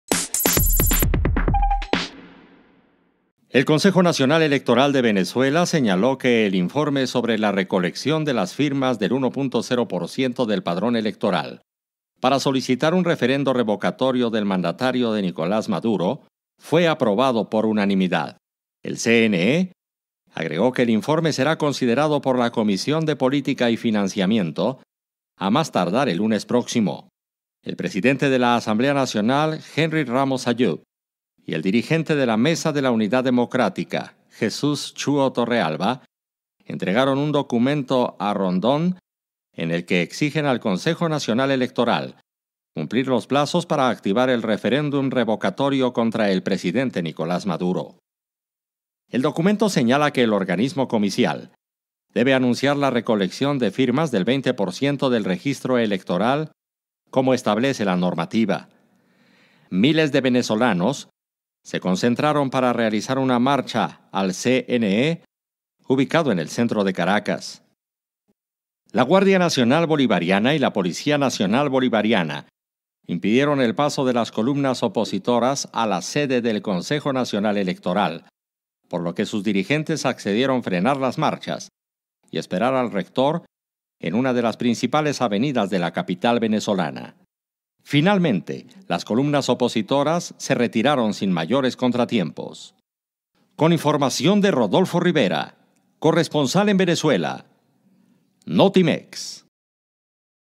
Aprueban recolección de firmas para referendo revocatorio en Venezuela
El Consejo Nacional Electoral de Venezuela señaló que el informe sobre la recolección de las firmas del 1.0 por ciento del patrón electoral fue aprobado por unanimidad